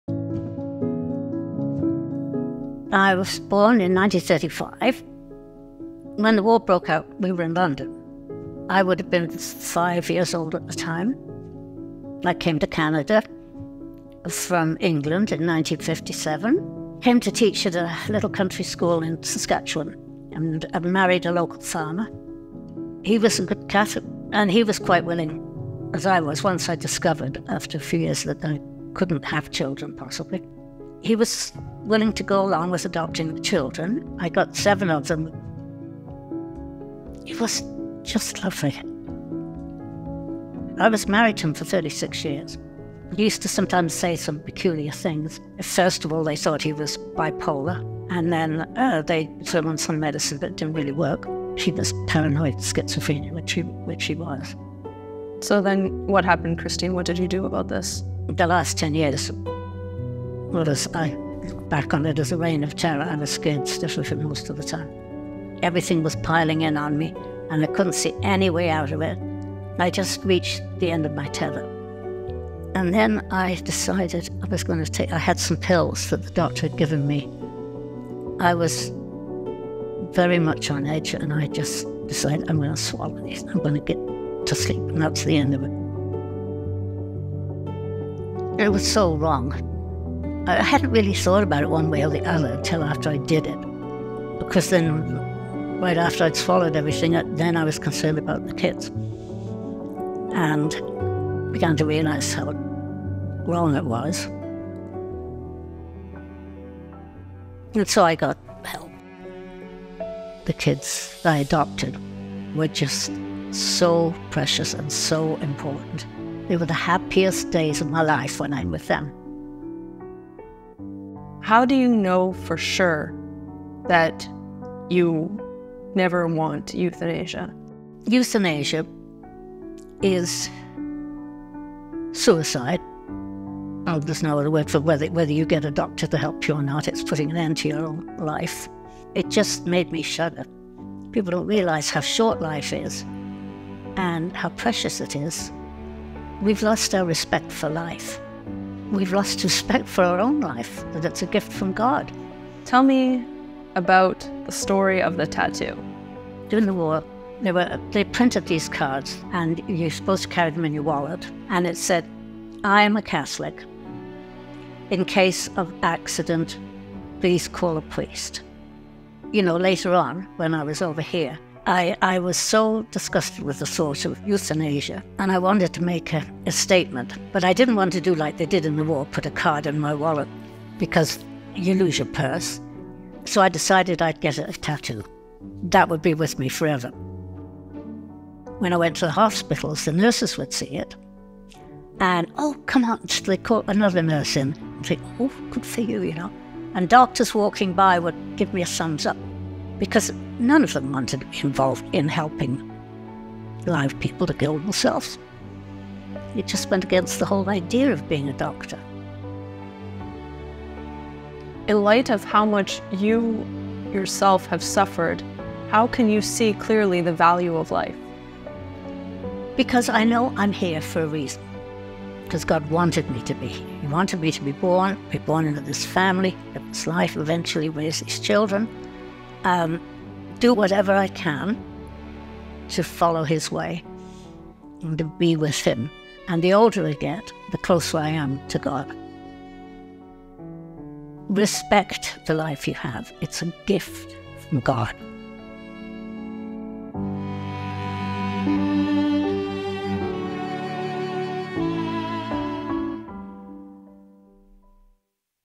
Non uccidetemi con l'eutanasia! In un tatuaggio l'amore per la vita di una donna canadese
Un film di Amanda Achtman.
Traduzione italiana di Chiara Bertoglio.
Ringraziamo di cuore l'attivista Amanda Achtman per questa sua commovente intervista e per il suo impegno a favore della vita.